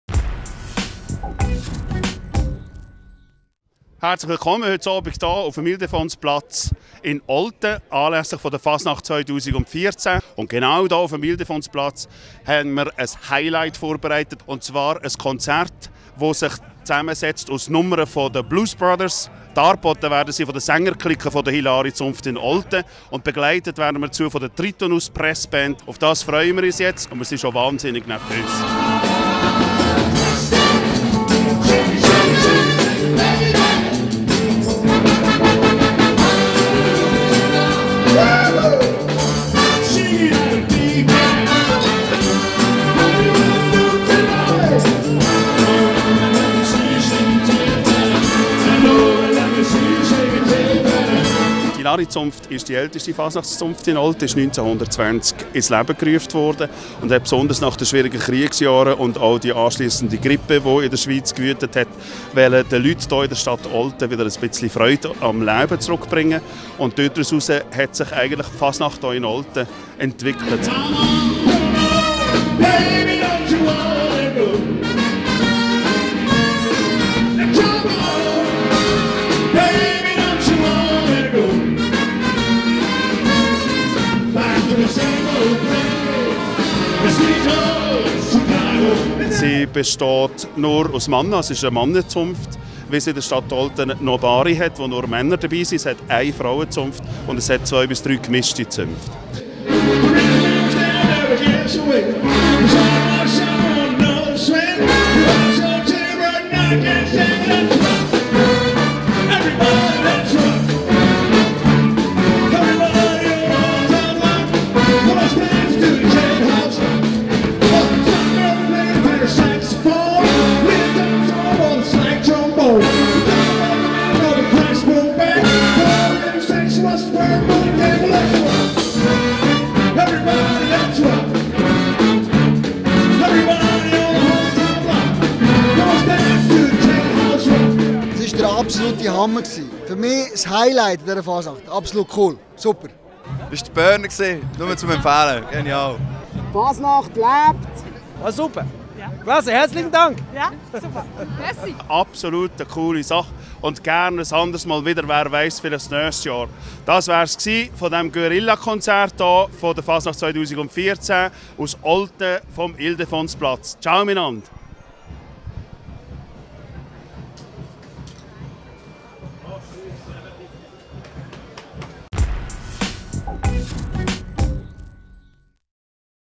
Guerilla-Konzert Olten, Hilari Zunft
Das Guerilla-Konzert der Sängerclique Hilari Zunft hat an der Oltner Fasnacht den Ildefonsplatz zum beben gebracht.